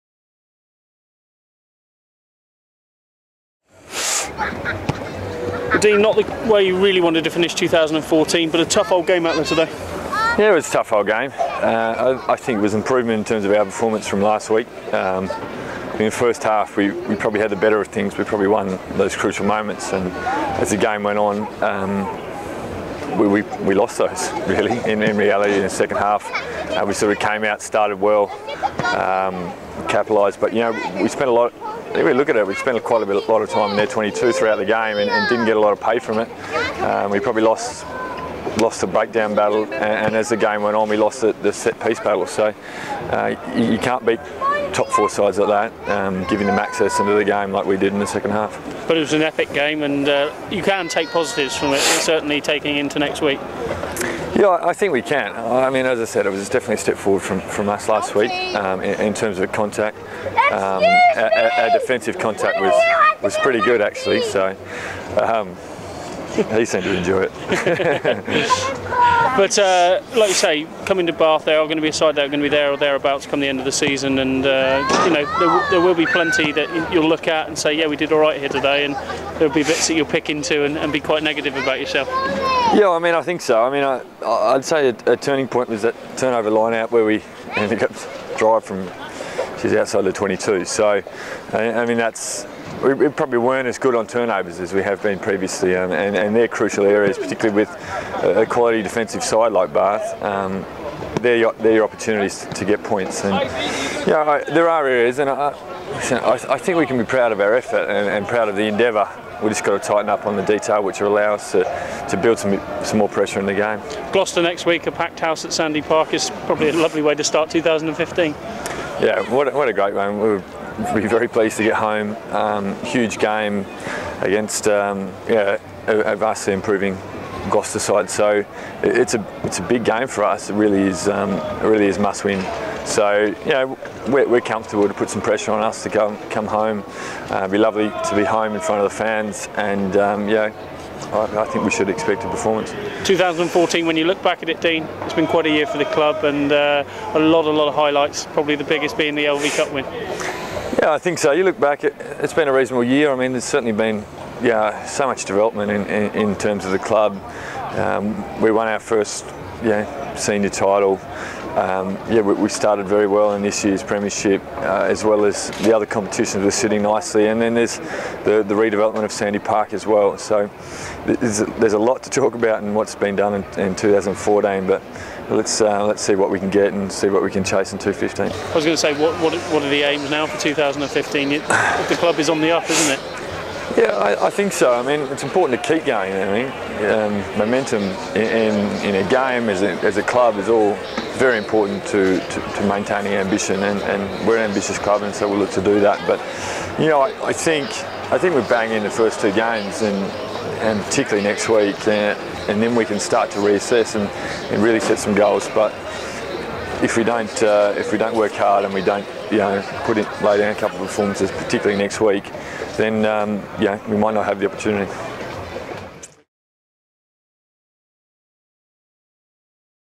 Chiefs TV - Dean Mumm post Bath
Chiefs TV - Dean Mumm talks pitch side at The Rec after the Chiefs were beaten by Bath Rugby in the Aviva Premiership...